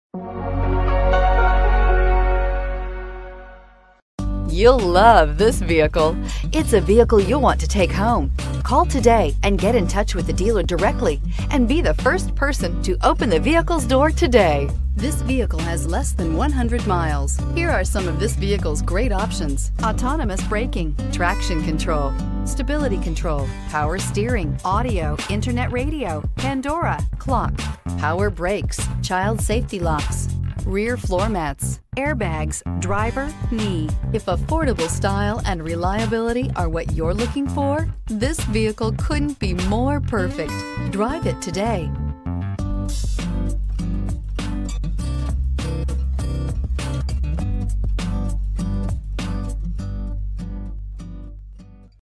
2019 LEXUS NX 300 F Sport for sale in Raleigh NC
2019 LEXUS NX 300 F Sport for sale in Raleigh NC - Stock # 91665
Year: 2019
Engine: 2.0L I-4 cyl
Transmission: 6-Speed Automatic
MPG Range: 22/28
Exterior Color: Ultra White
Interior Color: Circuit Red
Address:
VIN: JTJYARBZ3K2143075